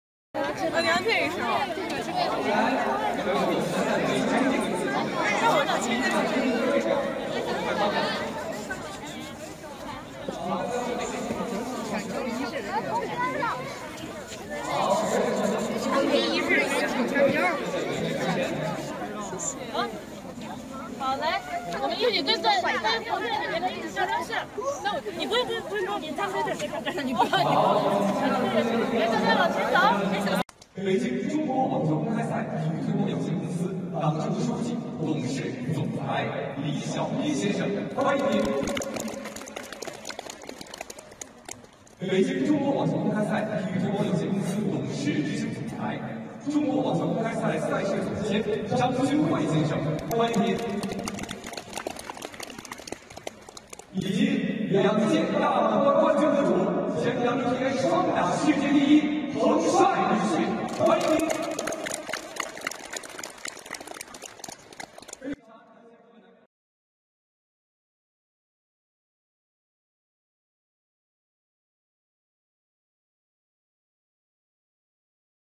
Missing Peng Shuai seen signing balls at Chinese tennis event
TENNIS star Peng Shuai has been pictured at a juniors' tennis event in Beijing after she was recently reported missing amid sexual assault allegations.

The 35-year-old recently vanished after she made an allegation of sexual assault against former senior Chinese vice premier Zhang Gaoli.

The former doubles world no. 1 hadn't been seen since November 3 after releasing a post detailing the allegations on social media.

And a prominent figure within the Chinese state media quickly claimed that Peng Shuai would 'show up soon', following public outcry about her disappearance.